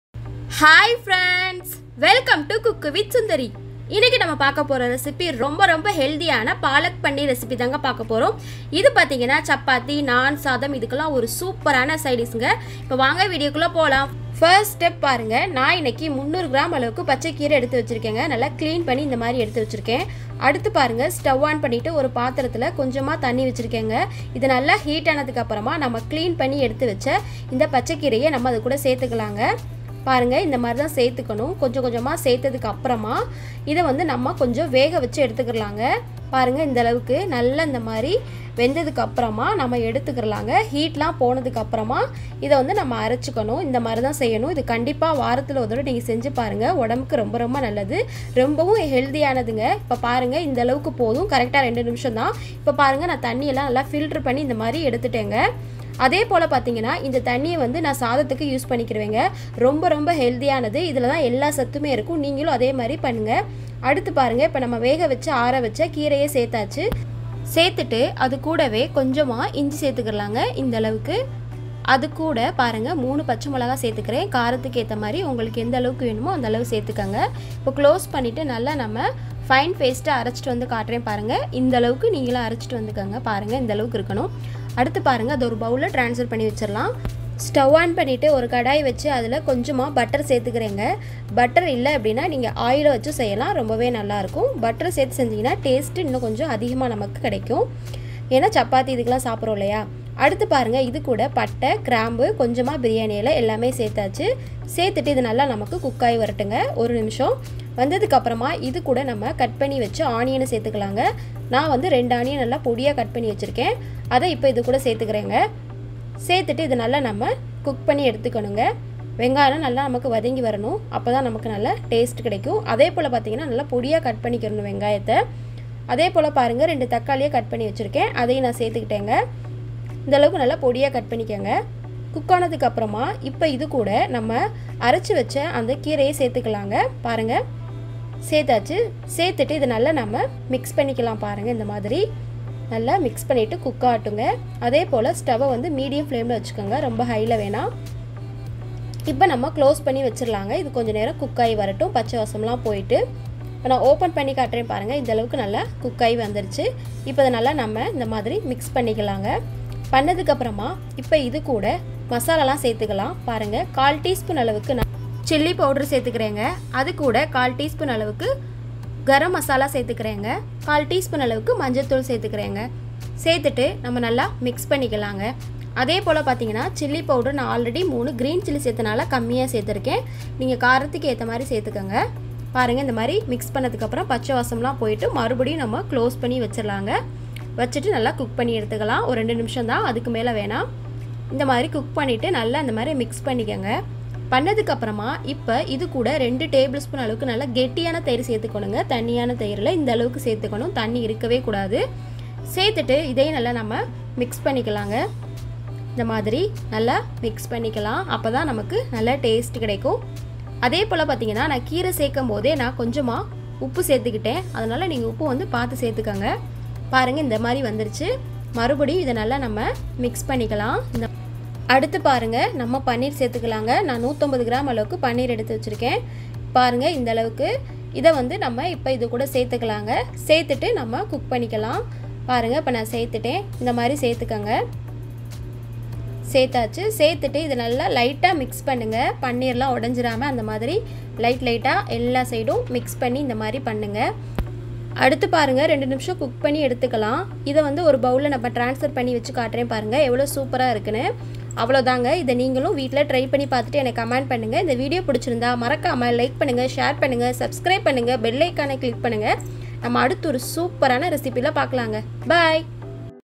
Palak paneer recipe || How to make palak paneer recipe in tamil || cook with sundari
Ingrediants

palak - 300 gm
ginger - 1 pcs
green chilli - 3 pcs
butter - 1 pcs
paneer - 150 gm
clove , cardamom , patta
required amount of oil and salt.

please do the recipes in your home and give me your valuable comments.
support COOK WITH SUNDARI.